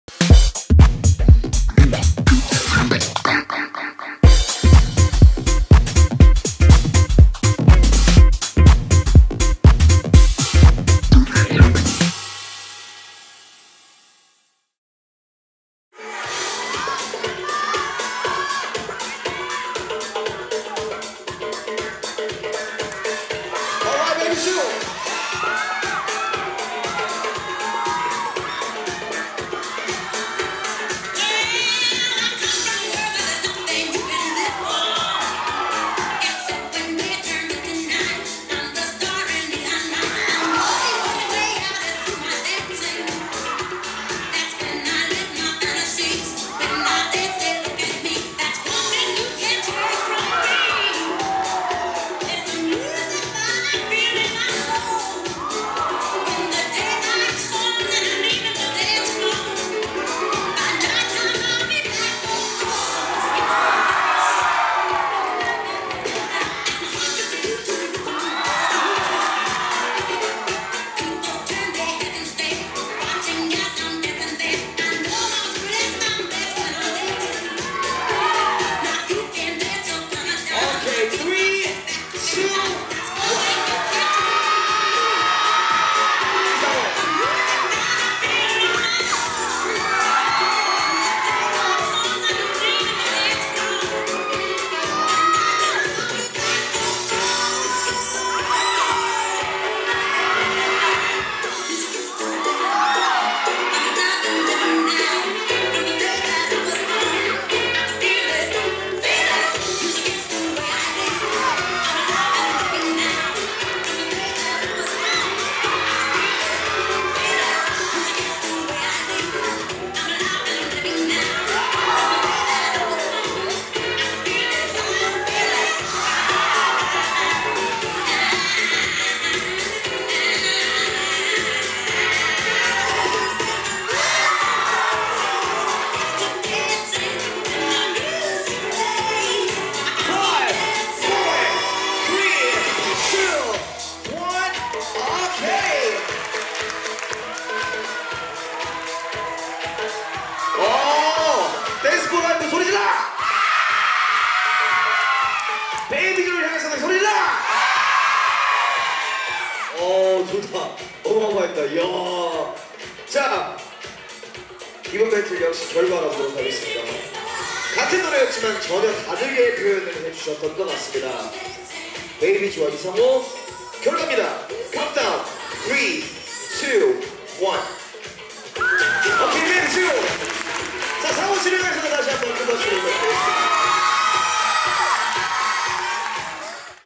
BABY ZOO vs MACAUㅣWAACKING Round of 16 ㅣ2019 LINE UP SEASON 5
2019 LINE UP SEASON 5 ANNIVERSARY

Round of 16 ㅣBABY ZOO (w) vs MACAU

💎WAACKING
🎧DJ
•SOM
👑JUDGE
•HANS •LIP J •WACOON
♟BATTLEGUEST
•PEANUT •WAACKXXXY •DASOUL •EL J

2019/5/11 - 12
@ARCO CONERT HALL(57PARTY TOWN)_GWANGJU CITY

Present by BITGOEUL DANCERS